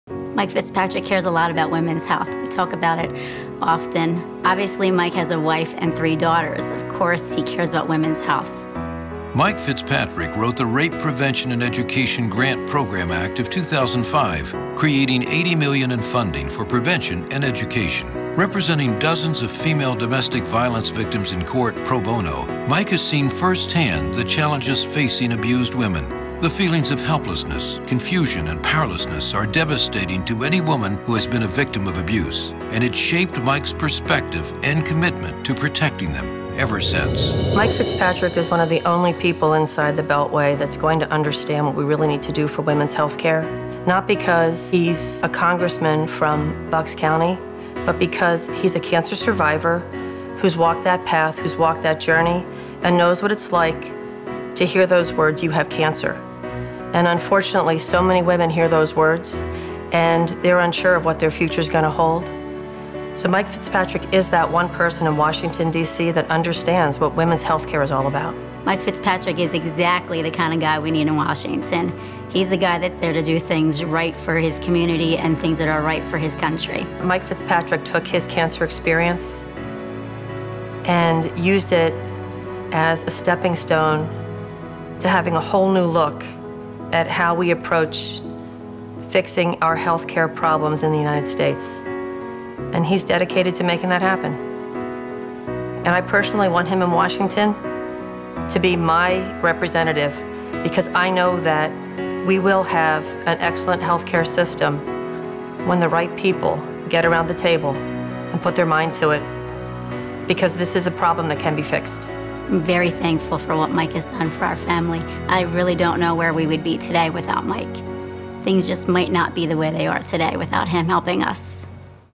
Fighting for Women
Hear two women's perspectives on Mike Fitzpatrick's efforts for women's health.